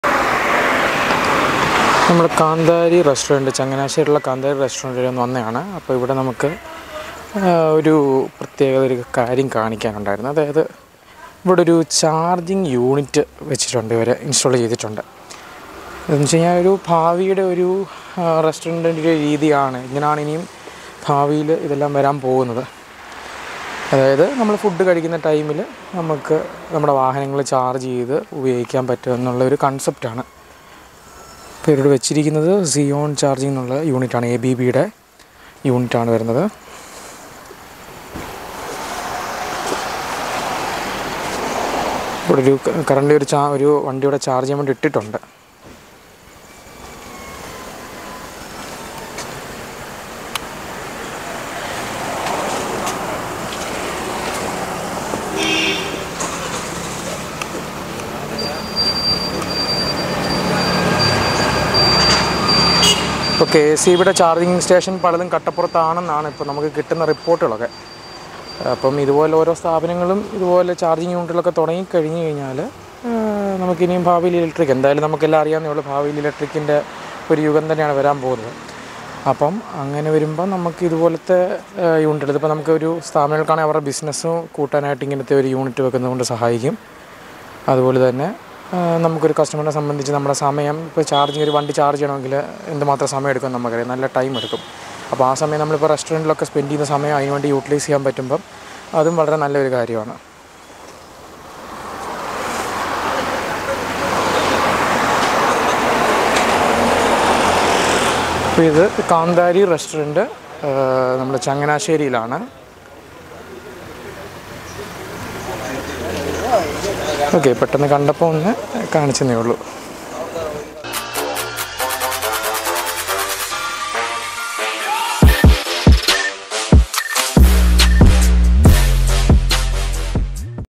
EVകൾ വ്യാപകമാകുന്നു.ചാർജിങ് സ്റ്റേഷനുകളുടെ സാന്നിധ്യവും വളരുന്നു | Charging station at restaurant
Follow me on
and don't forget to turn on the BELL ICON